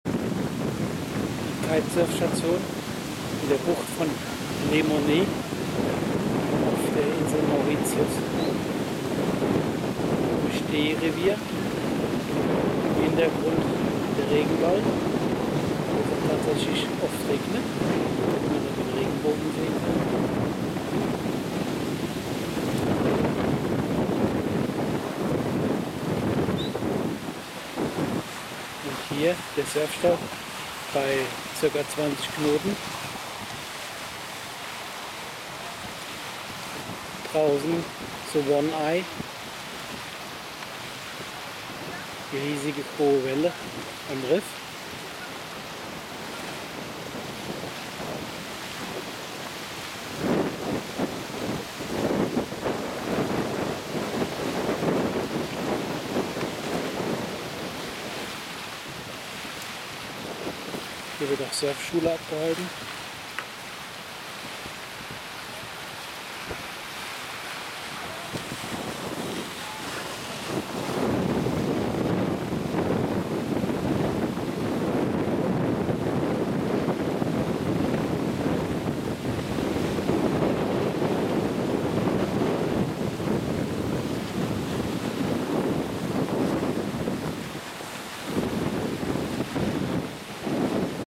Mauritius - Le Morne (Kitesurfen)
Kitesurf Lagune bei Le Morne auf der Insel Mauritius im indischen Ozean